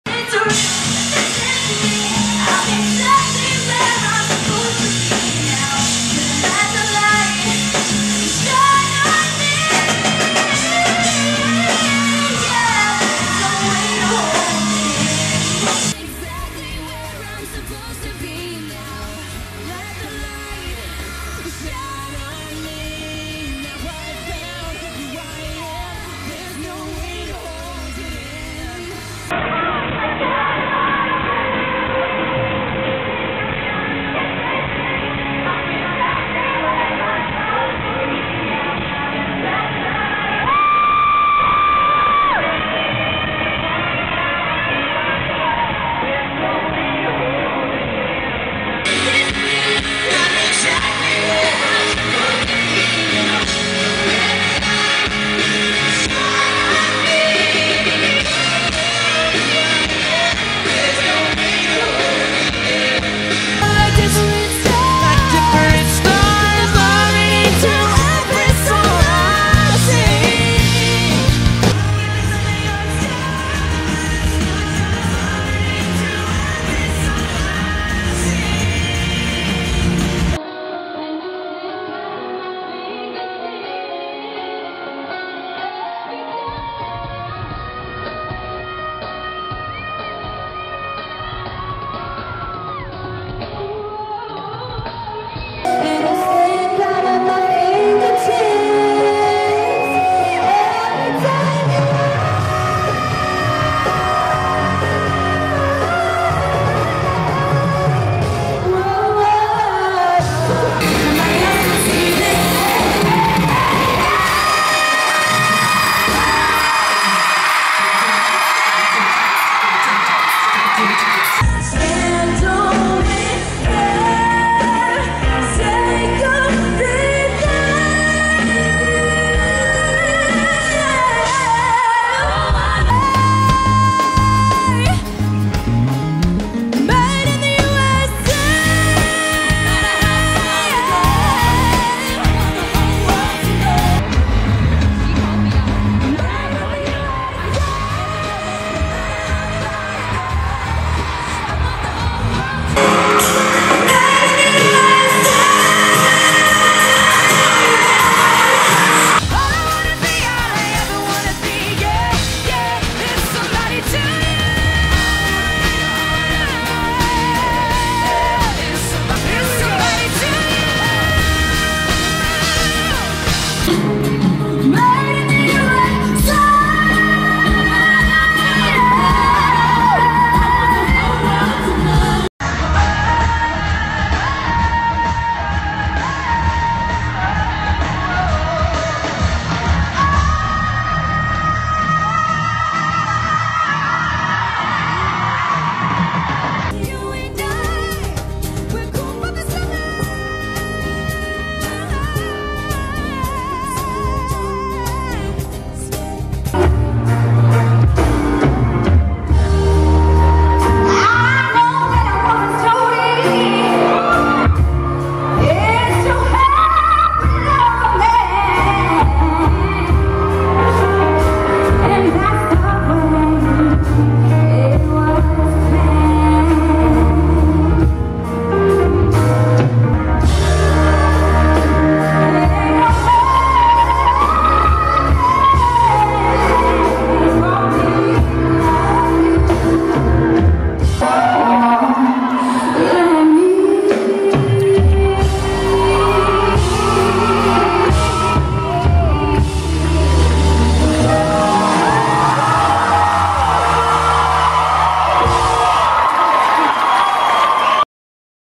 Demi Lovato: Eb5 Belt Evolution (2008-2015)
Just a fun idea I had. Let me know your favorite Eb5!